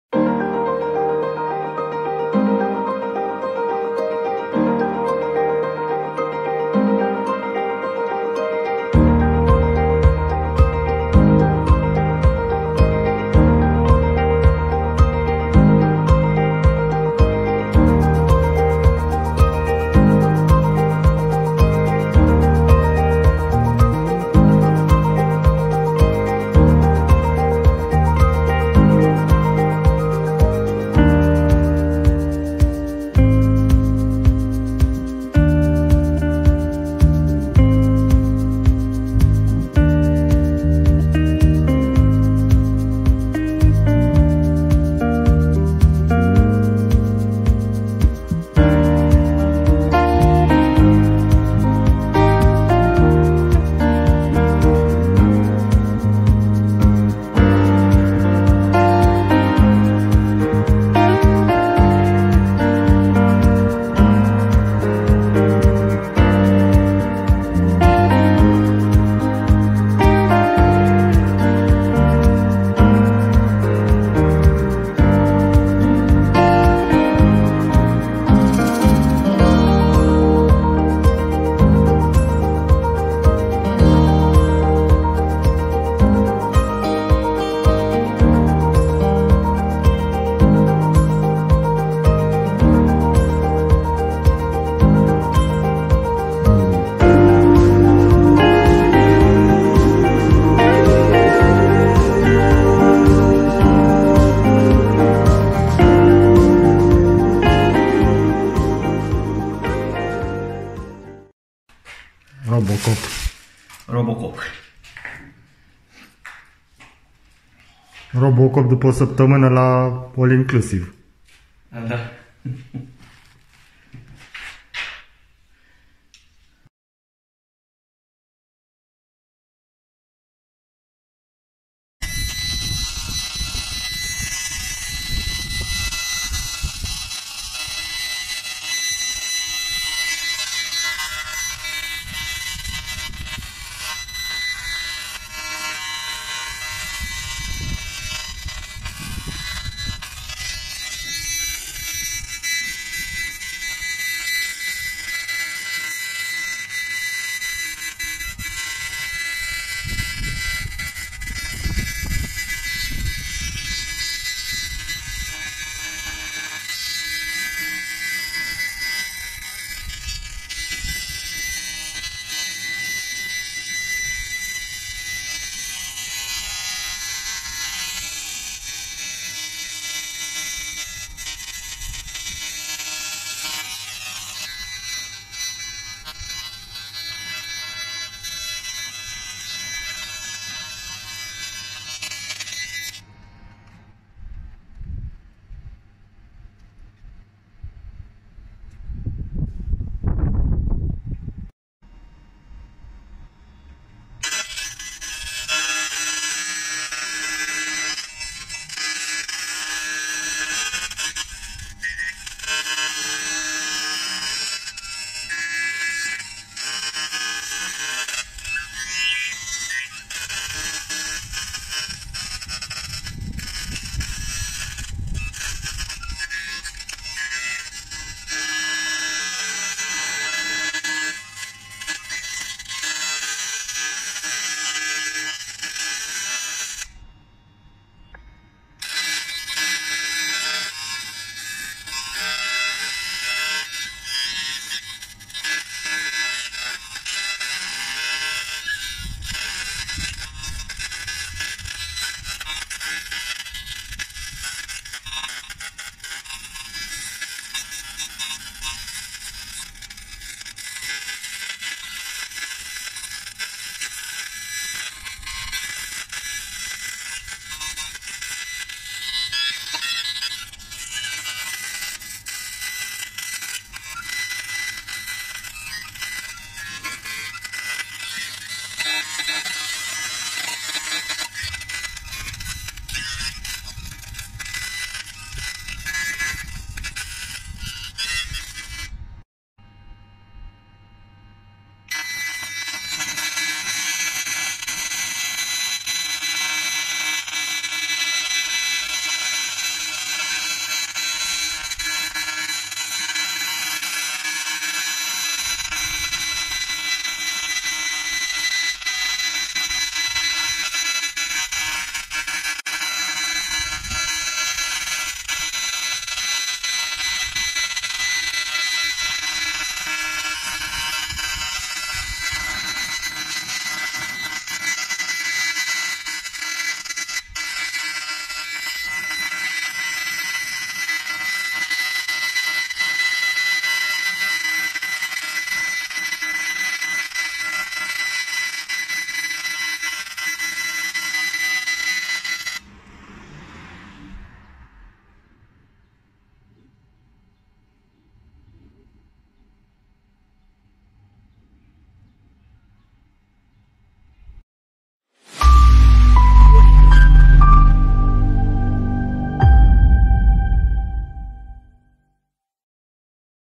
Romanian client received AKQ-1500 and put it into operation, very nice -Shine AccTek Machinery China
AKQ-1500 AccTek 1500W Fiber Laser Cleaning Machine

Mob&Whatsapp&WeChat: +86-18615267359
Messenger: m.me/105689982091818